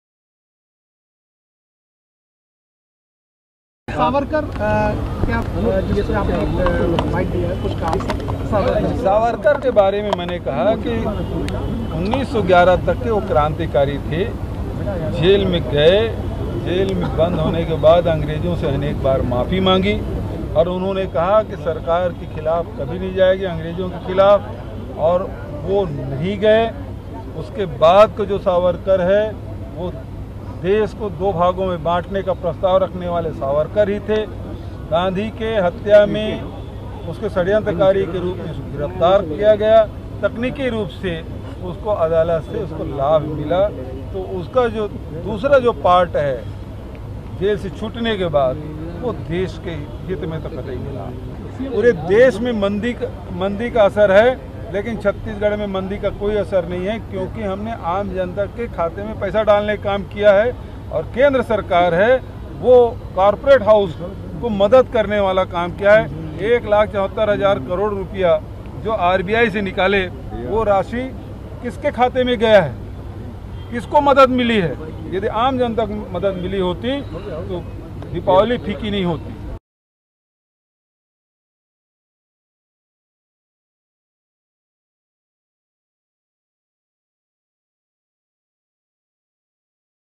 Savarkar was the one who put forward proposal to divide country: Bhupesh Baghel
Raipur (Chhattisgarh): Chhattisgarh Chief Minister Bhupesh Baghel said that Vinayak Damodar Savarkar was the one who put forward the proposal to divide the country and he was arrested as one of the conspirators in Gandhi ji's murder. "Savarkar was a revolutionary till 1911, after being jailed he sought forgiveness from British many times.